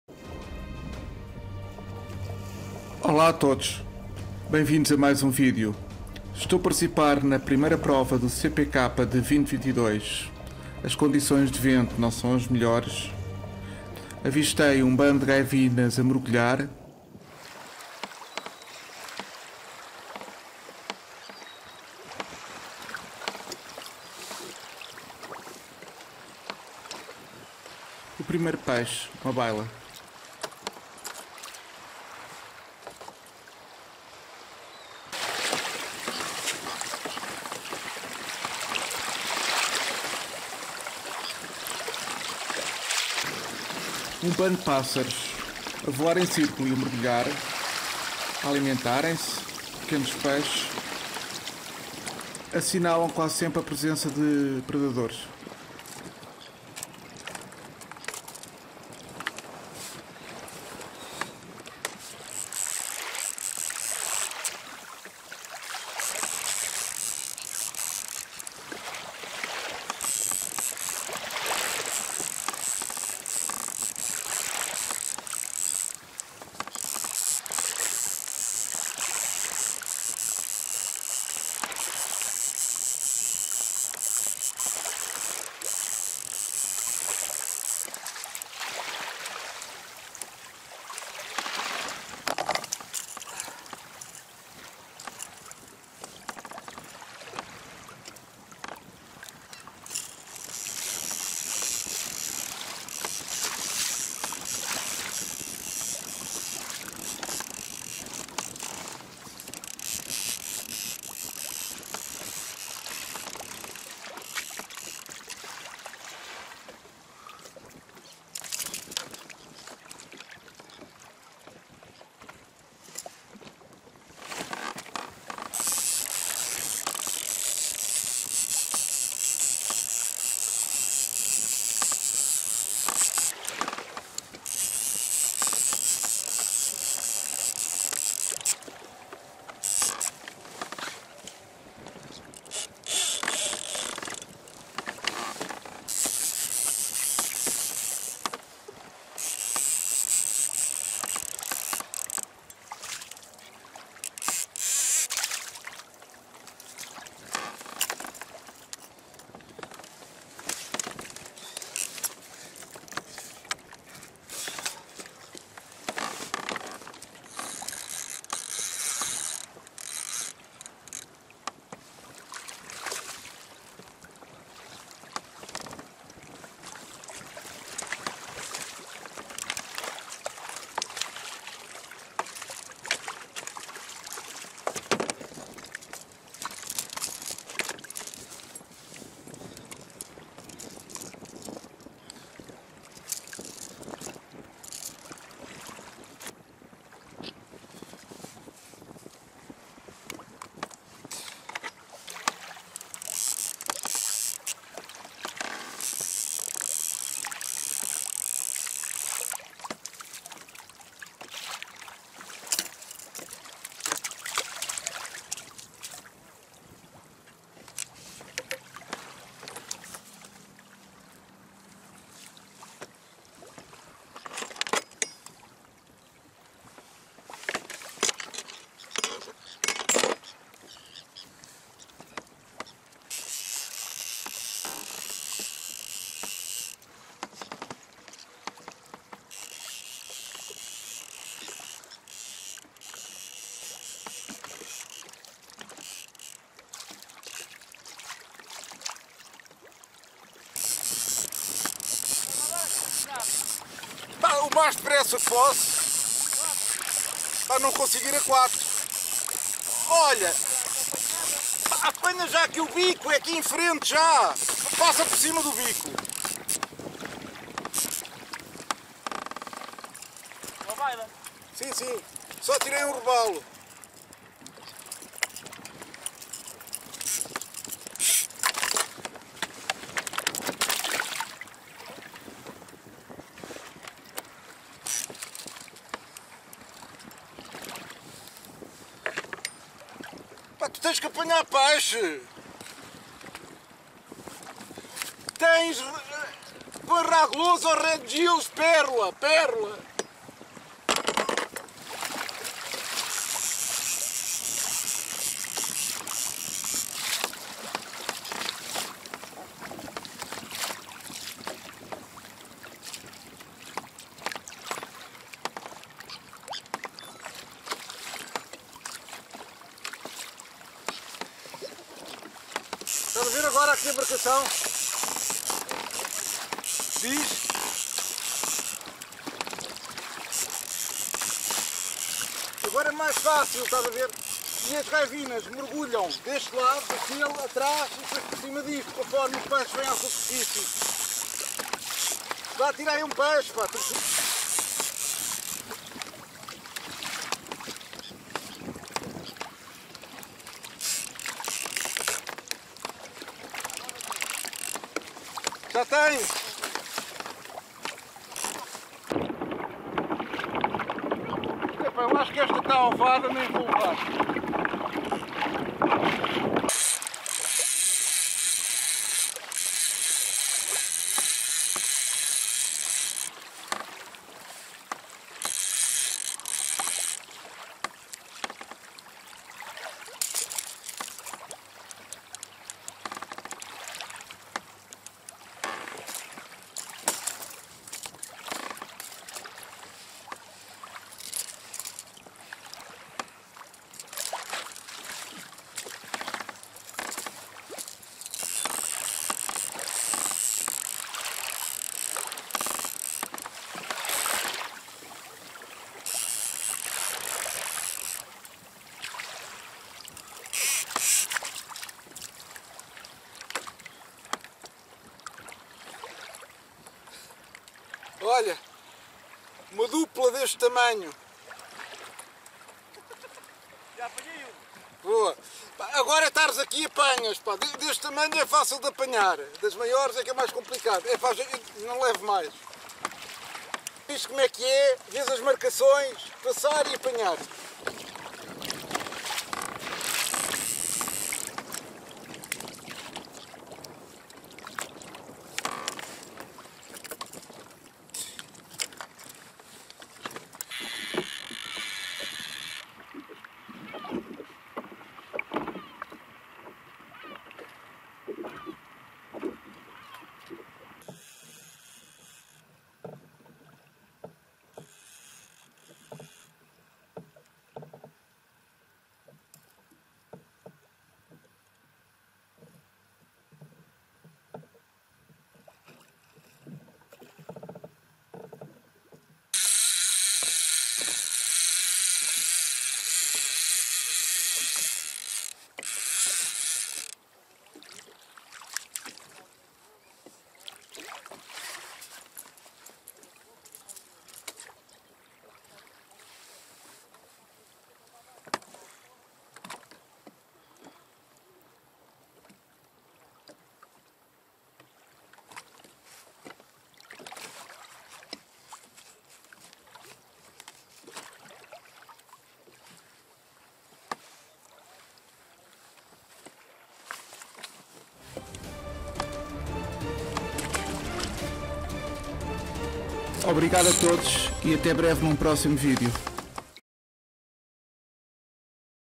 Using Birds to Find Fish - Seabass and Spotted Seabass - Flock of Terns
Flocks of birds feeding in the sea are the best indicator of the presence of fish, a fisherman should always be aware of birds to obtain the best results.

Kayak fishing during the CPK I open 2022 tournament, the strong wind made fishing conditions harsh.
The rules of the fishing federation allowed to score an unlimited number of fish, promoting a senseless killing of relatively small specimens, from a certain point I started to release the fish, despite everything I ended up in the second position.

Subtitles available
Thank you very much and see you soon in the next video!

Portugal
27th March 2022
Nuno Morão